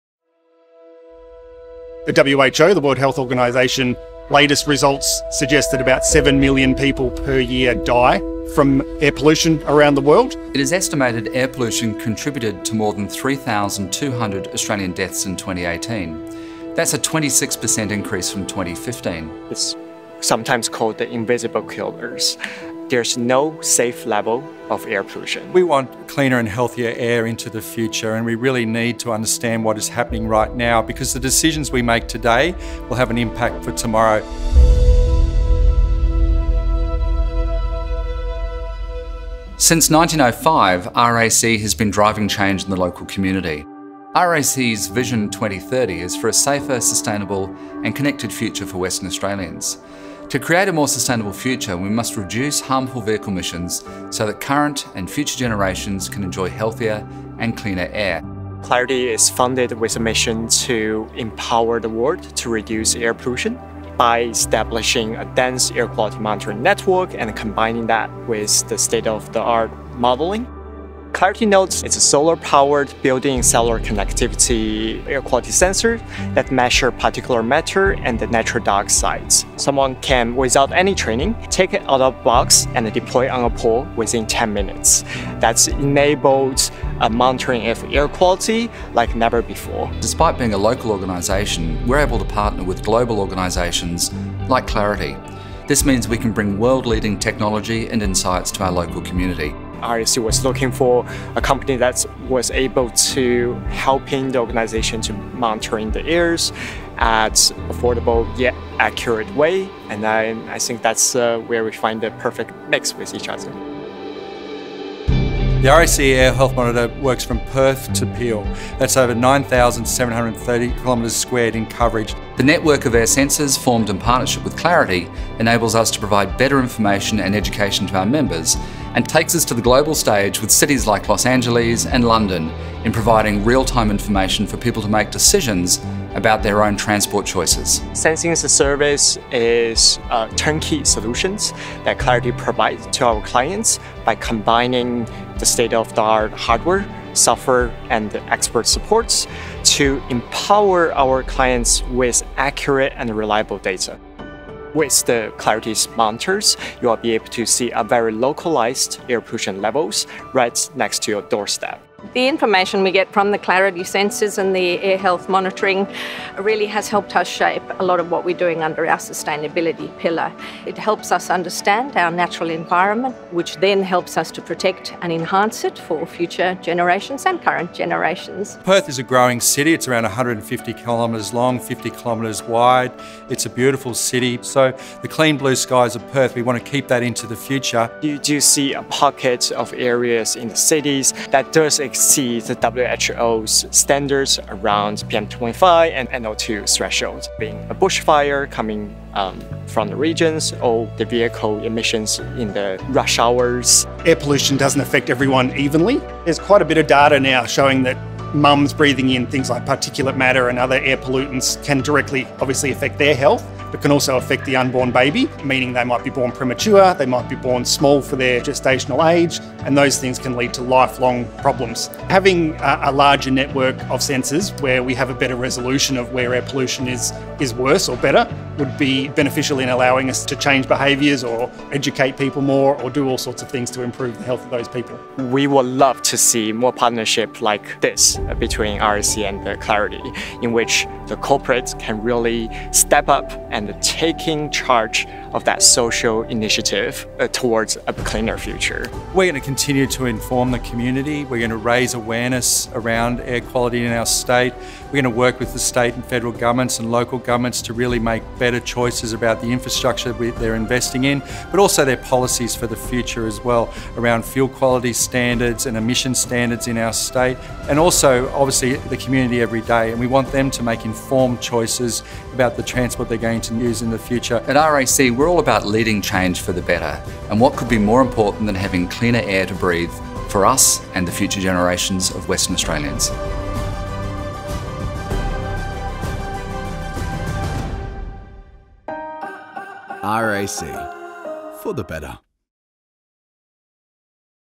RAC Air Health Monitor: Moving towards a cleaner future
The RAC Air Health Monitor is the biggest air sensor network in Australia. Powered by hundreds of sensors, showing hour-by-hour changes to the air quality across the metropolitan area.

Hear from our team on how we're helping move towards a future of cleaner transport.